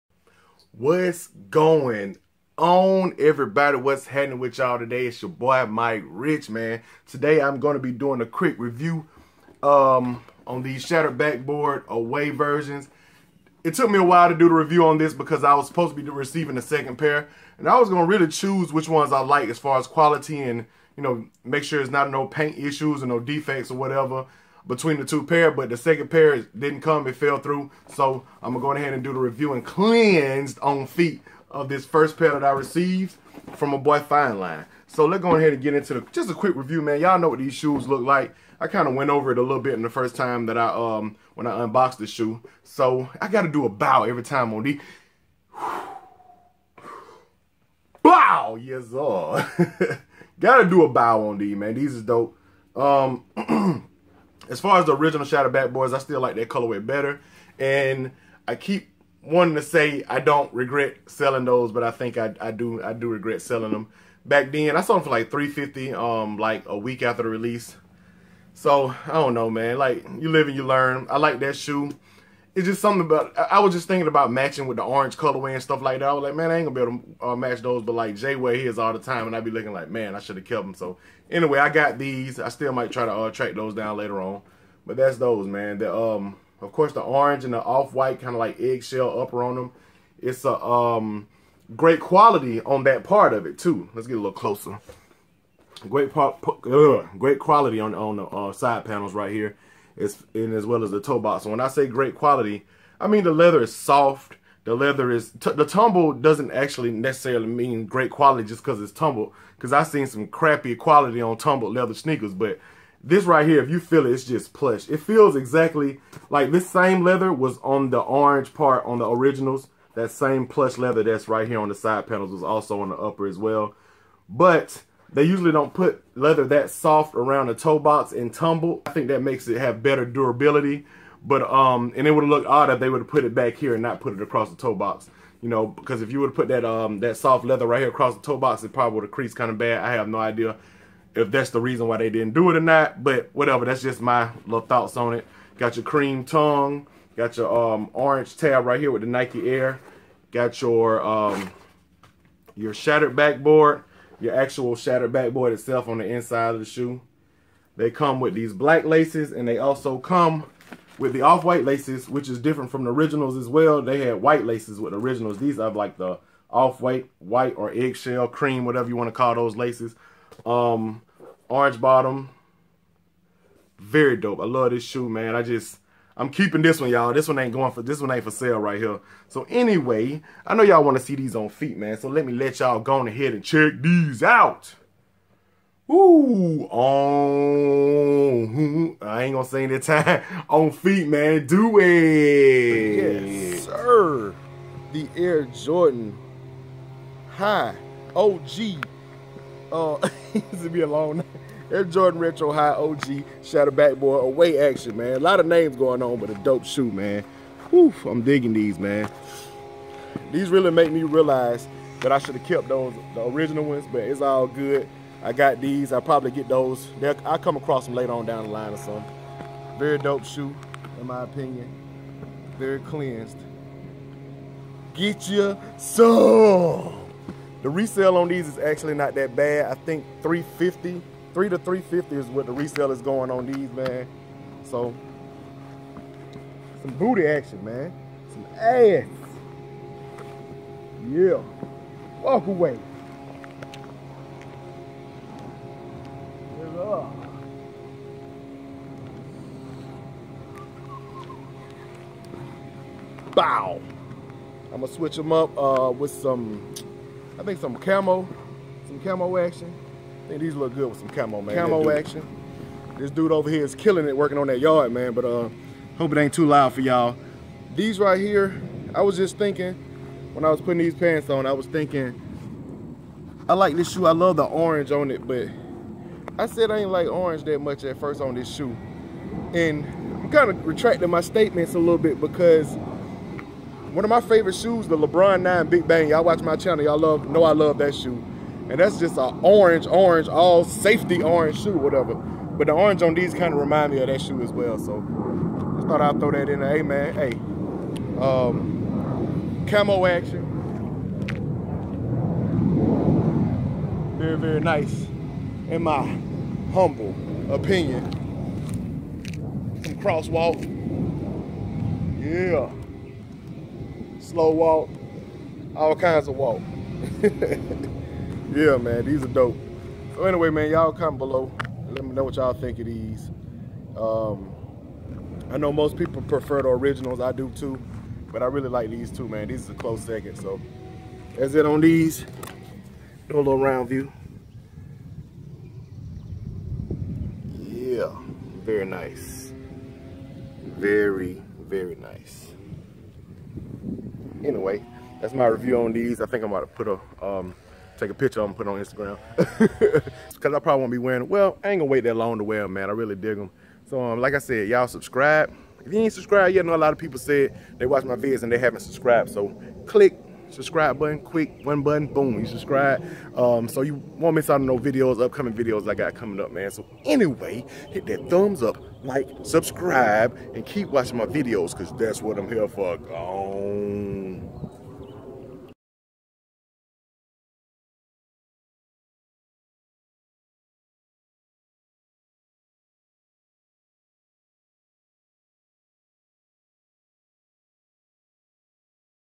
JORDAN 1 SHATTERED BACKBOARD "AWAY" REVIEW AND ON FEET!!!
YOU CAN SEND STUFF TO:
MIKE RICH